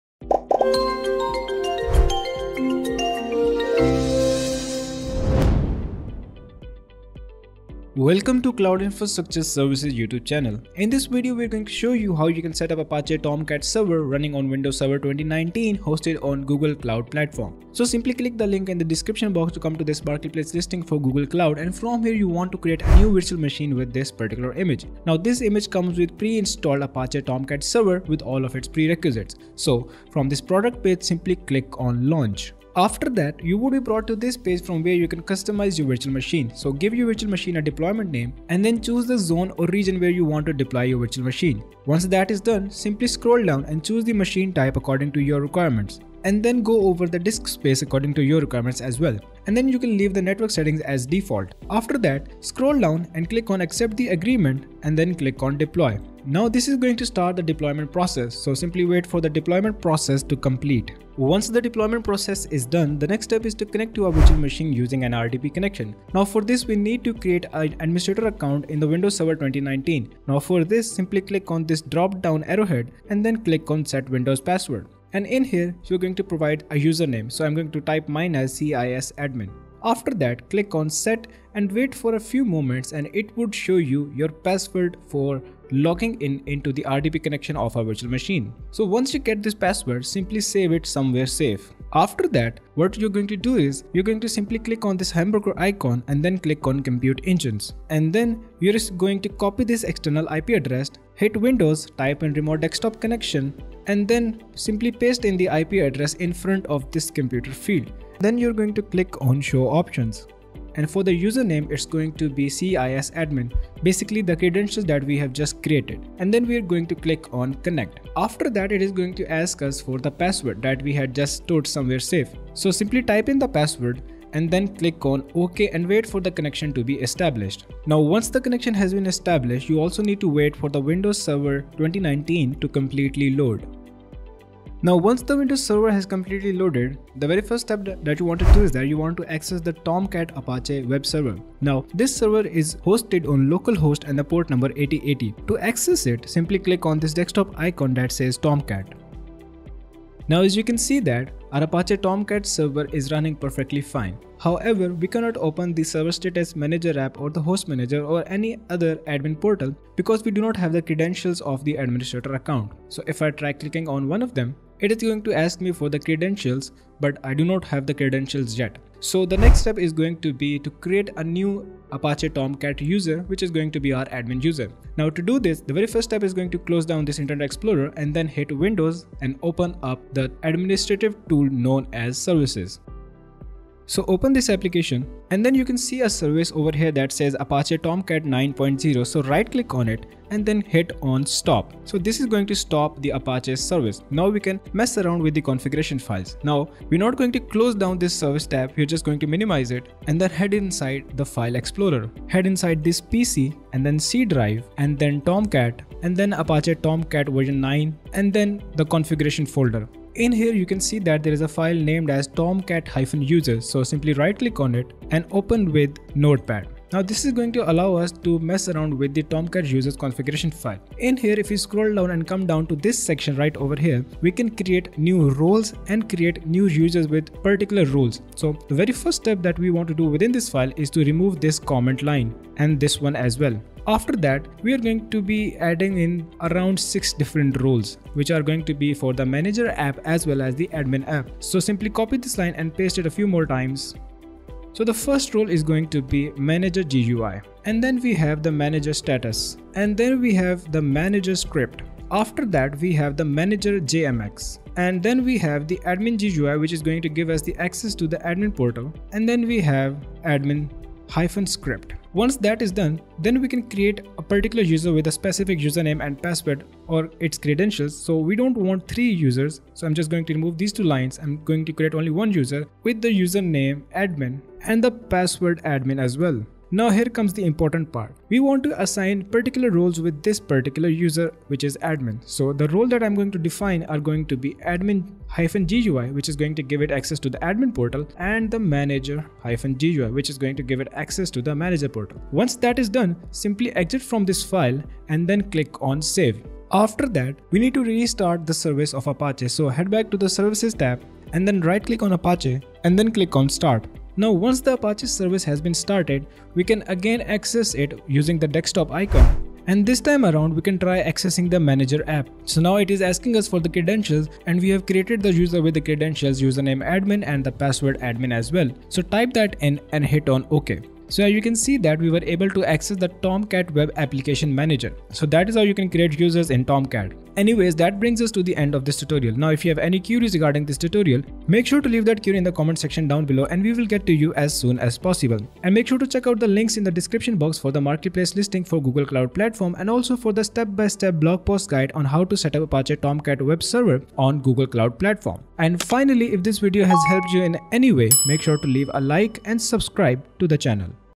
How to Setup Apache Tomcat on Windows Server 2019 in GCP
How to Setup Apache Tomcat on Windows Server 2019 in GCP : The Apache Tomcat® software is an open source implementation of the Java Servlet, JavaServer Pages, Java Expression Language and Java WebSocket technologies. The Java Servlet, JavaServer Pages, Java Expression Language and Java WebSocket specifications are developed under the Java Community Process.

This video will show how you can setup Apache Tomcat on Windows in GCP

⏰Timestamps⏰
00:00 Intro to setup Apache Tomcat Windows in GCP
00:23 Creating Tomcat Windows Server
01:04 Establishing RDP connection
02:18 Accessing Apache Tomcat
02:49 Creating Tomcat Users
05:09 Accessing Tomcat Admin\Manager App
05:27 Outro of Setup Apache Tomcat on Windows Server 2019 in GCP

Apache Tomcat Features:
-Lightweight application that loads fast & redeploys fast
-Very flexible
-Suite of extensive, built-in customization options
-Extremely stable platform to build on
-Can be installed behind an additional firewall for added security
-Runs independently of the Apache installation